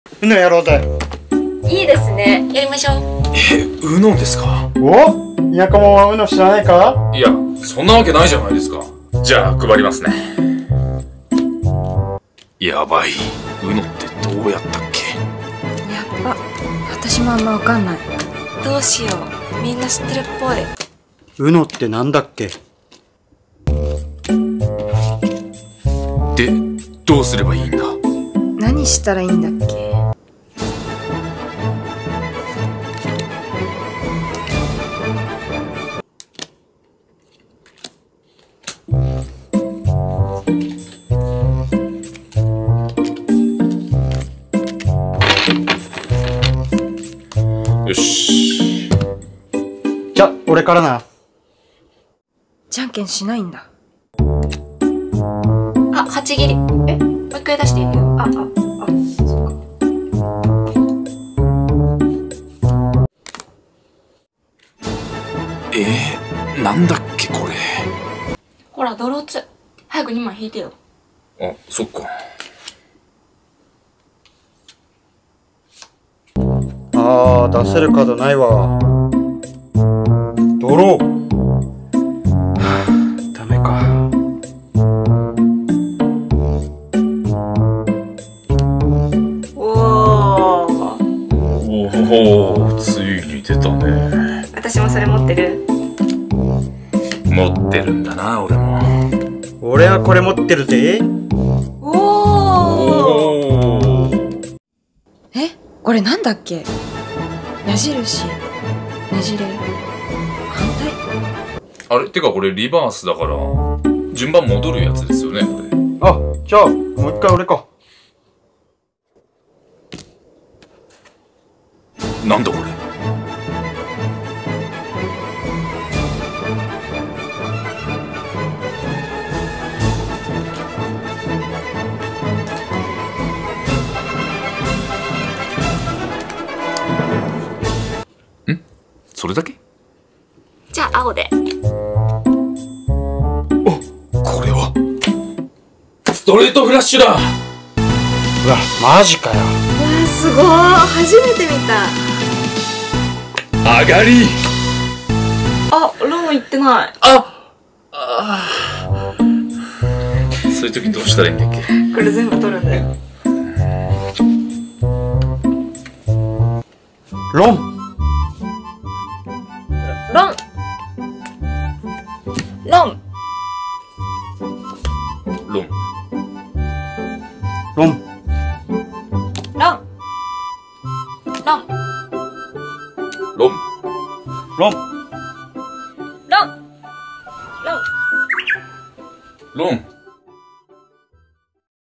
『4人でウノ』昼休みにみんなでUNOやったけど誰もルール知らない
昼休みにみんなでUNOやったけど誰もルール知らない

ーーー